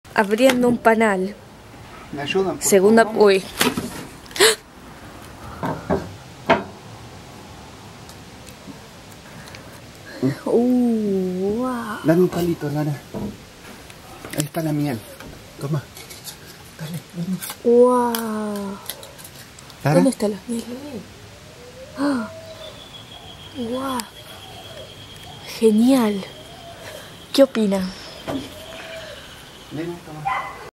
Colmena de Tetragonisca angustula en La Plata Argentina
Tetragonisca es un género de himenópteros ápidos de la tribu Meliponini; son de las abejas sin aguijón más comunes. Tetragonisca angustula (Latreille, 1811), popularmente conocida en Portugués como jataí, y en castellano como yateí, es un forrajeador generalista muy eficiente nativo de la flora de América Central y Sudamérica. Se distribuye desde el sur de México al norte de Argentina. No fue encontrado en la cordillera de los Andes, pero si en la Caatinga Brasilera y en las regiones del nordeste de Brasil y en regiones amazónicas.